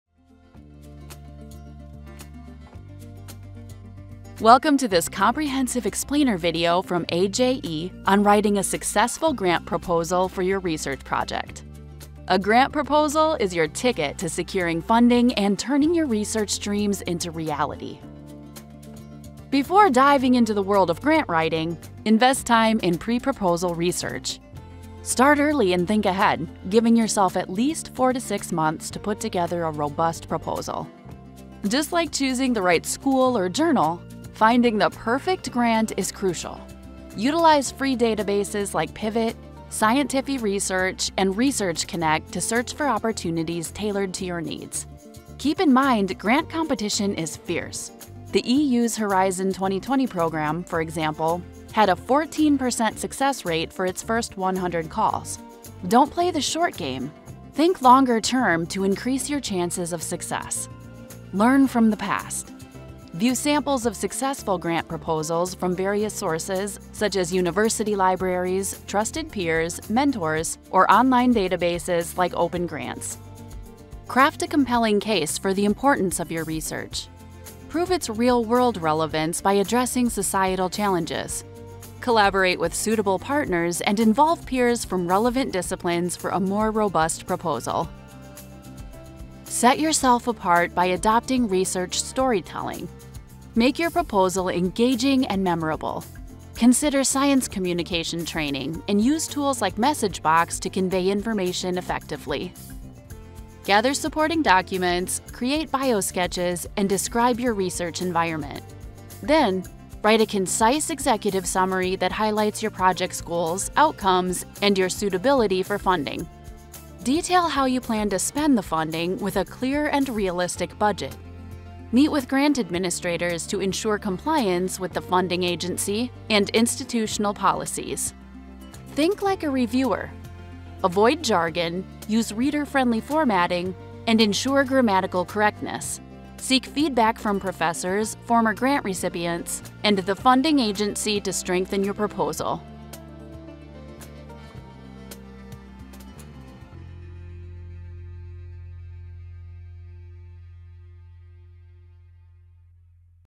How to Write a Winning Grant Proposal | AJE
Securing grants requires strategic planning. Identifying relevant opportunities, building collaborations, and crafting a comprehensive grant proposal are crucial steps. Watch our video on grant writing, finding grants, and applying for grants to get the funding for your research.